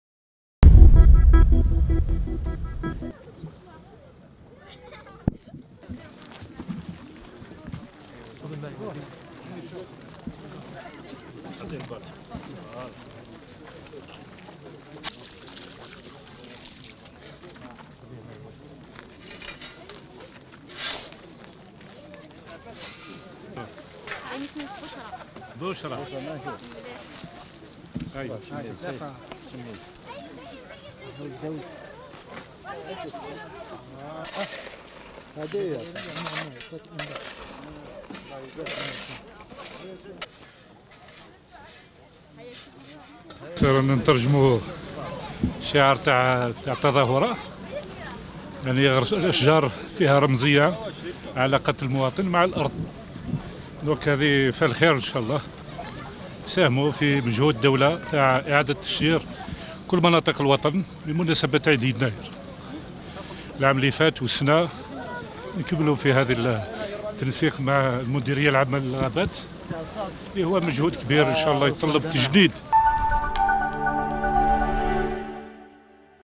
باتنة: غرس 1000 شجيرة بمناسبة الاحتفال برأس السنة الأمازيغية
باتنة(وأج) - تم غرس 1000 شجيرة ببلدية بوزينة، وهذا بمناسبة الاحتفال برأس السنة الأمازيغية.